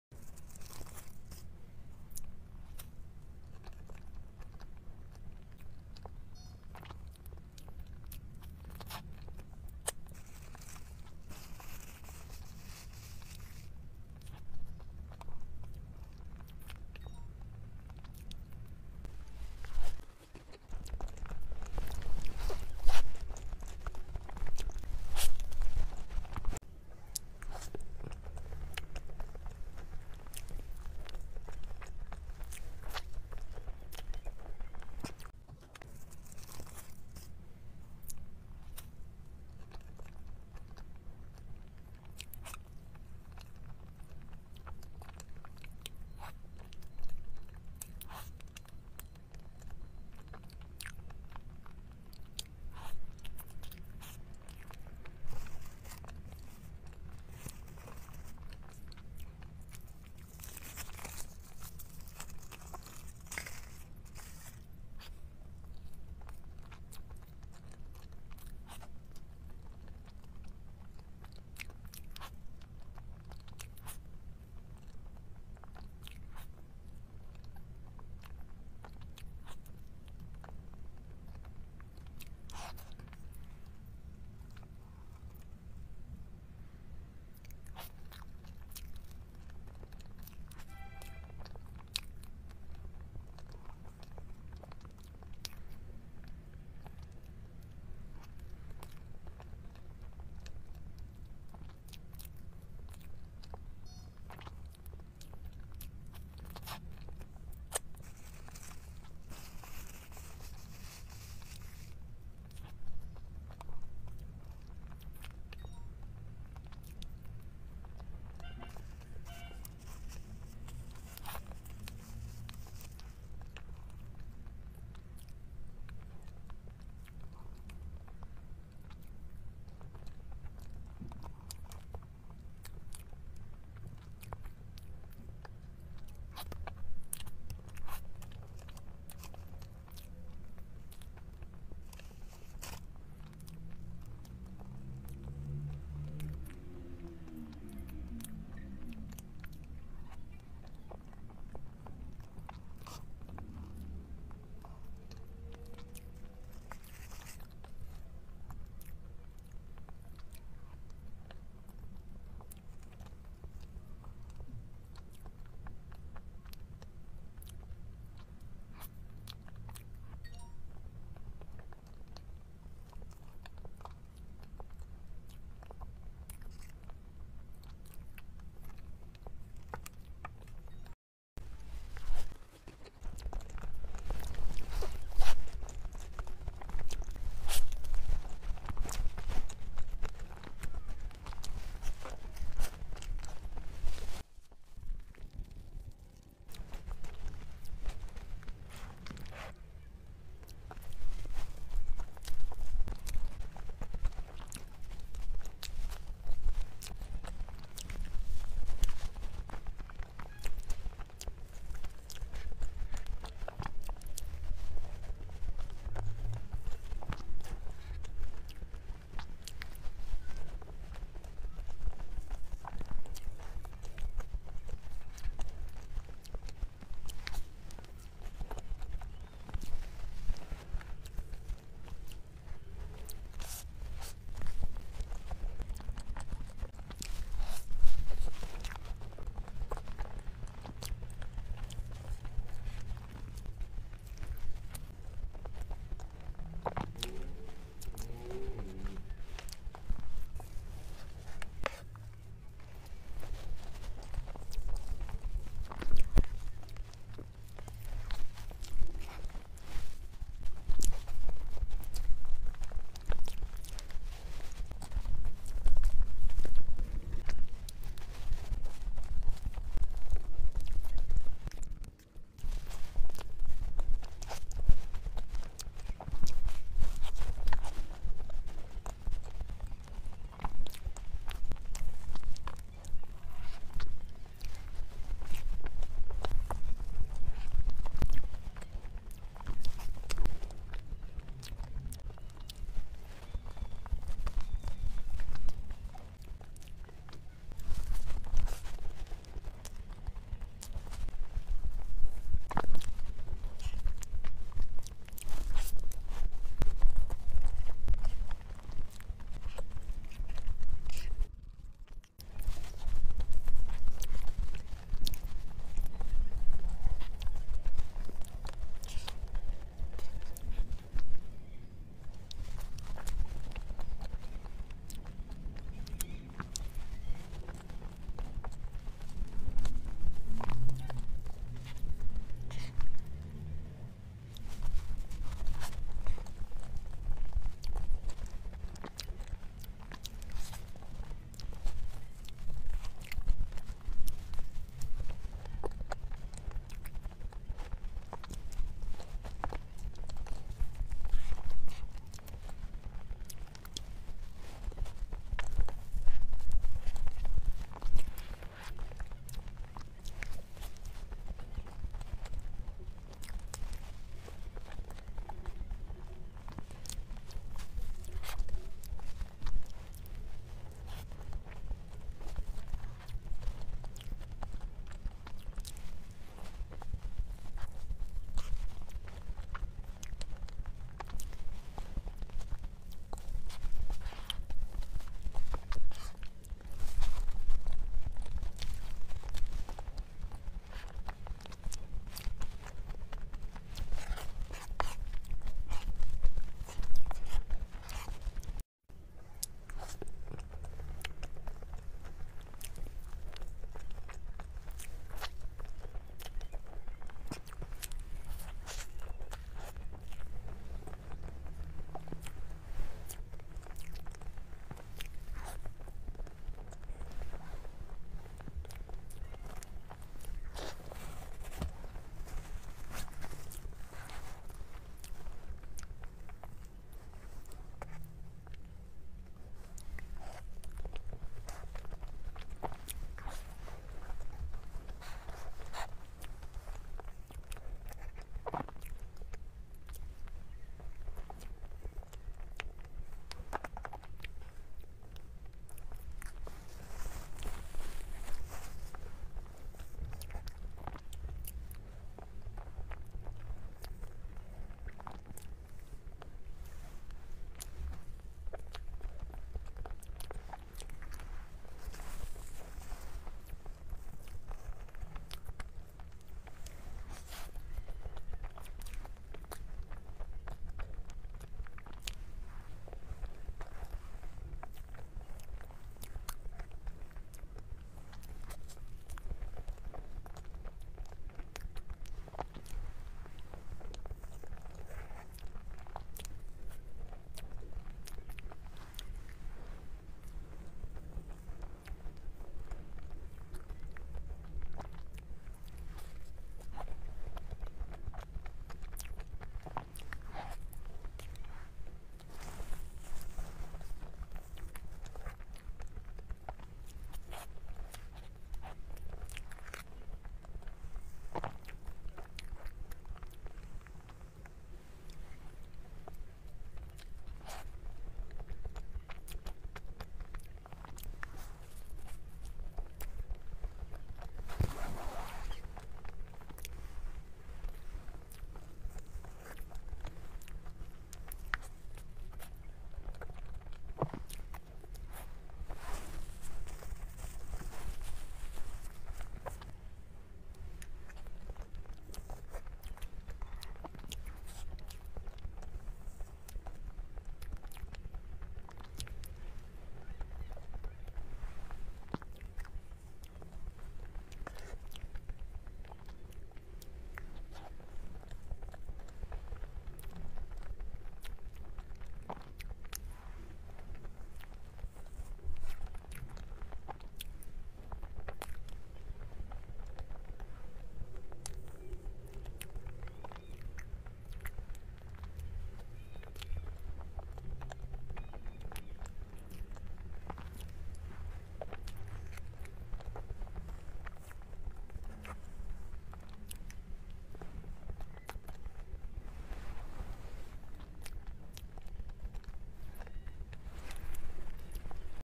Kwai Custard Crepe Cake Mukbang Compilation || Chocolate & Matcha Crepe Cake || ASMR Addict
Delicious Custard Crepe Cake Mukbang Compilation || Chocolate & Matcha Crepe Cake || ASMR Addict